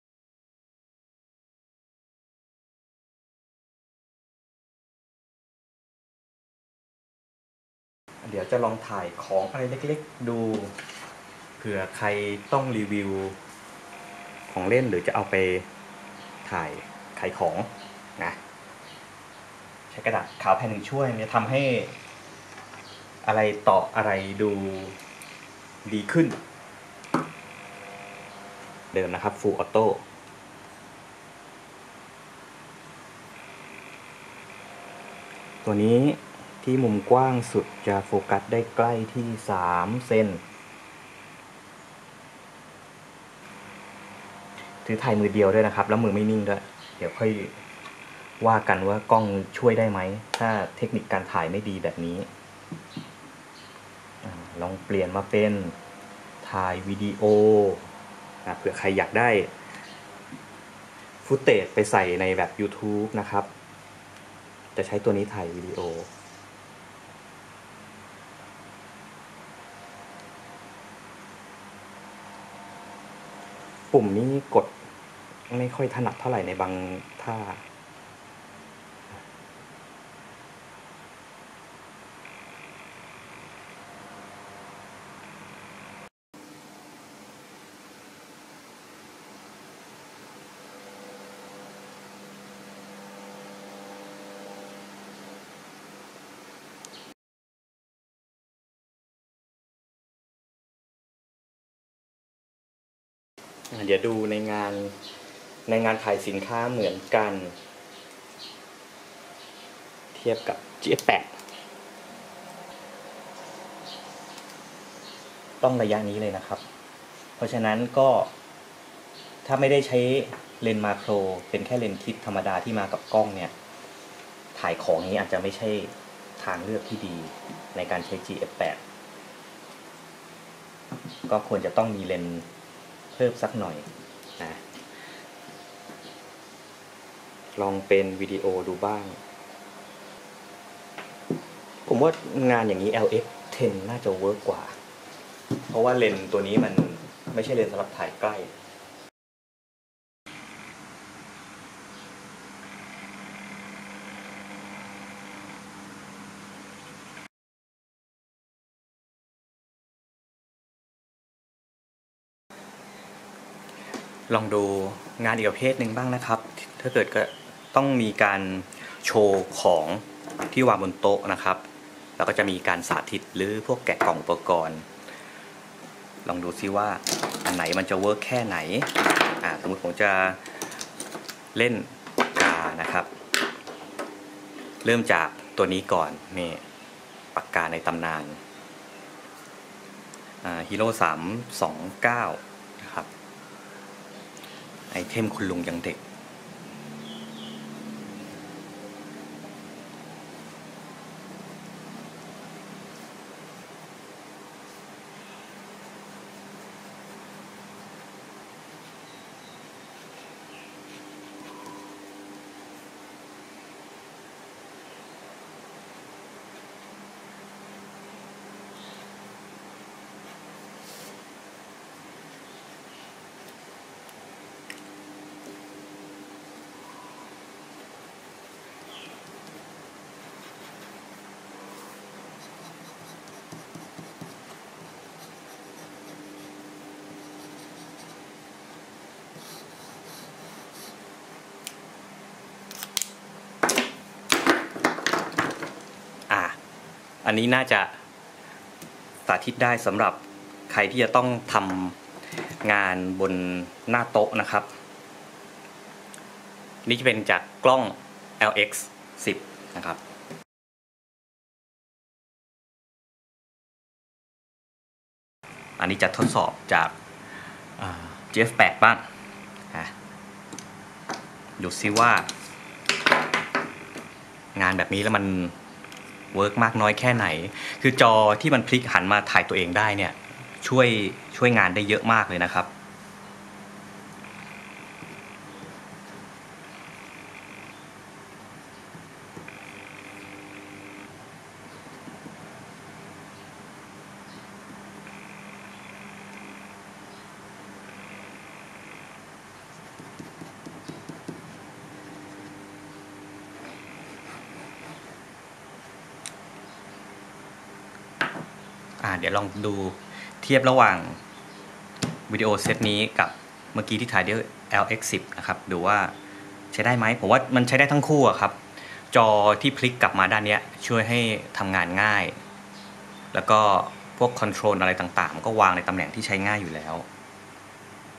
Panasonic Lumix GF8 & LX10: Sample 3 On the Table
ดูการทำงานเมื่อใช้เป็นกล้องสำหรับถ่ายสิ่งของบนโต๊ะ การรีวิวสินค้า หรือแกะกล่องสินค้า